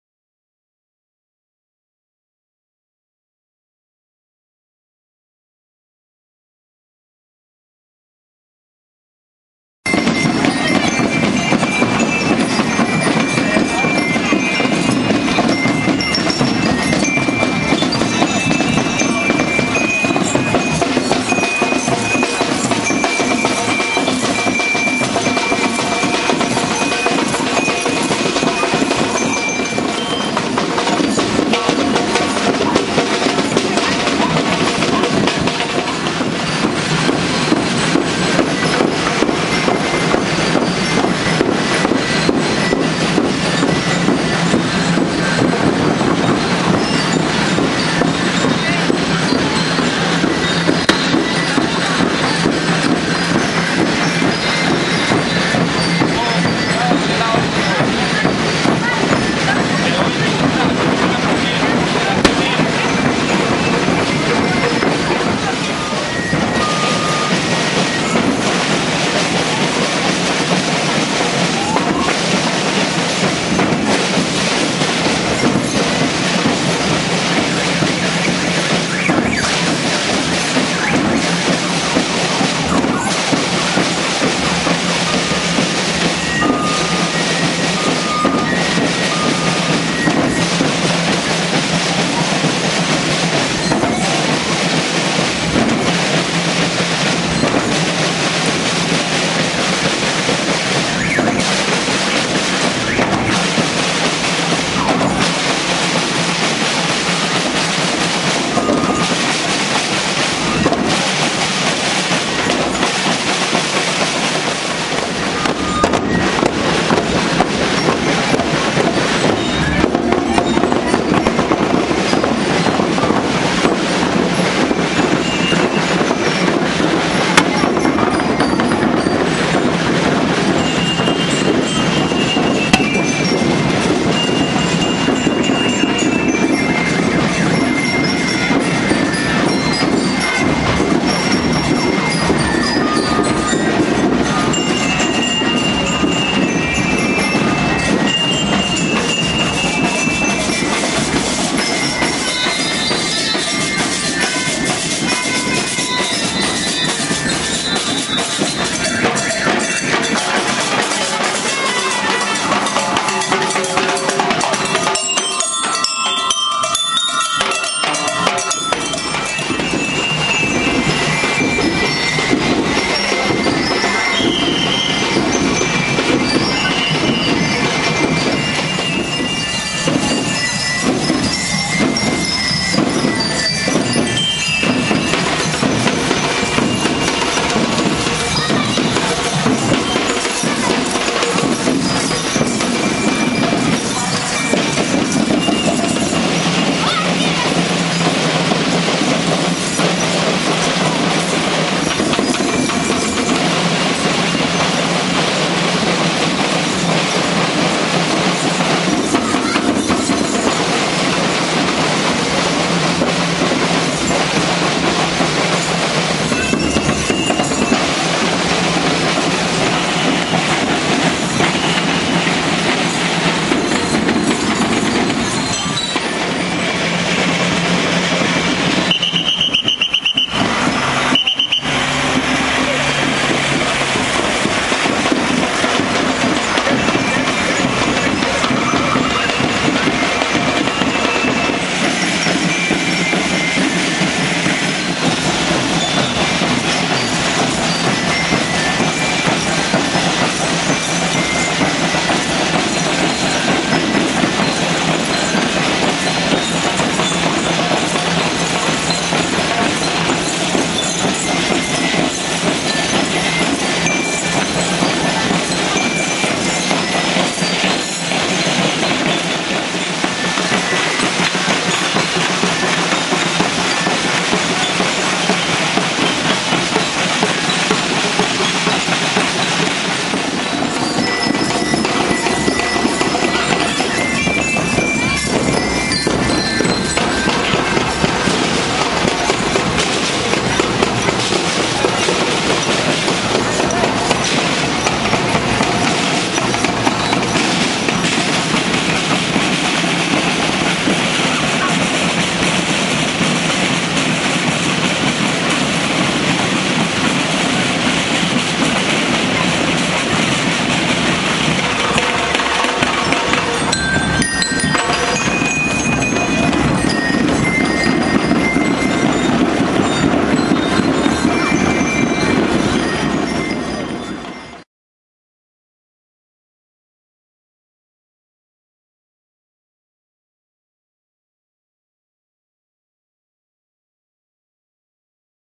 Matagalpa, Virgen De La Merced
Esto es parte del recorrido de la virgen de la mercdez de este 23 de septiembre del 2008